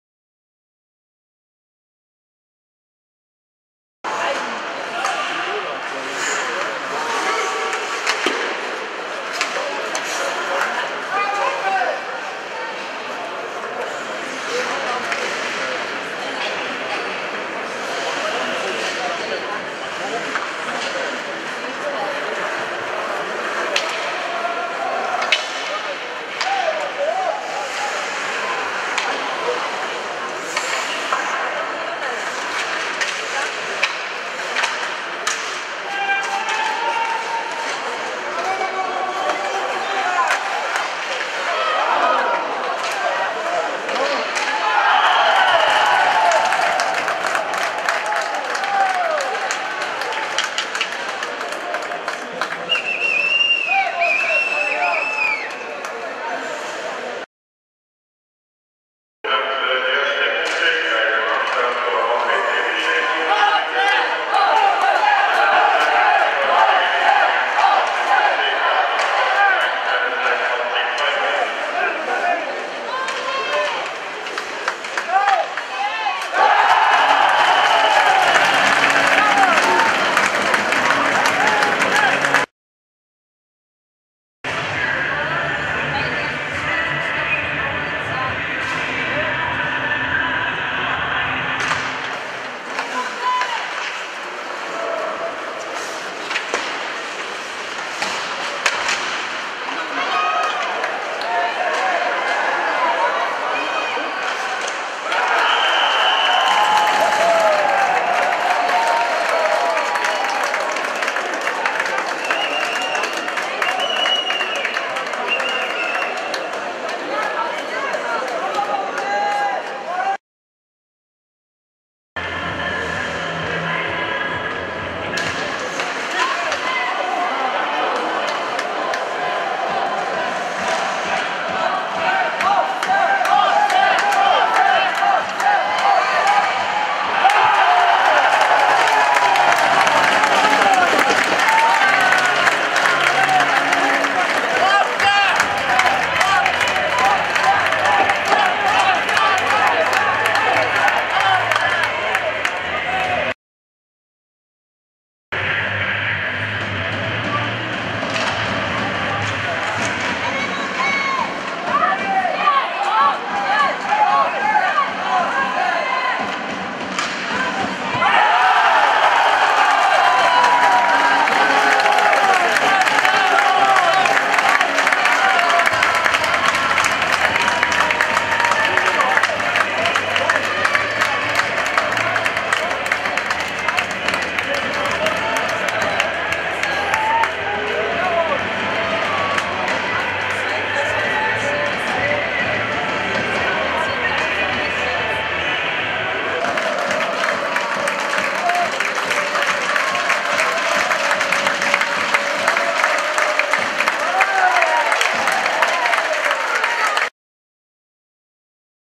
HC Sierre - HC Monthey 25.11.2014 3-2
Patinoire Graben Sierre 612 spectateurs
Victoire après prolongation